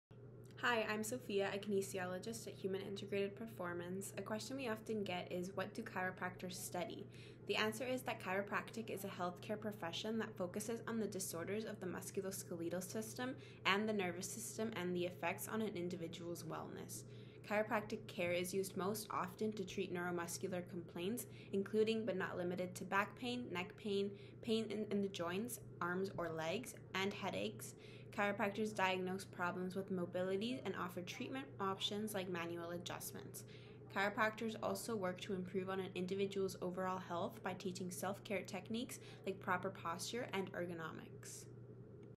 What do chiropractors study?
Our clinical team has started to answer your frequently asked questions!

Check out yeghip.com to read more of our common FAQs.

Question: What do chiropractors study?

Answer:
Chiropractic is a health care profession that focuses on the disorders of the musculoskeletal system and the nervous system and the effects on an individual’s wellness. Chiropractic care is used most often to treat neuromusculoskeletal complaints, including but not limited to back pain, neck pain, pain in the joints of the arms or legs, and headaches. Chiropractors diagnose problems with mobility and offer treatment options like manual adjustments. Chiropractors also work to improve an individual’s overall health by teaching self-care techniques like proper posture and ergonomics.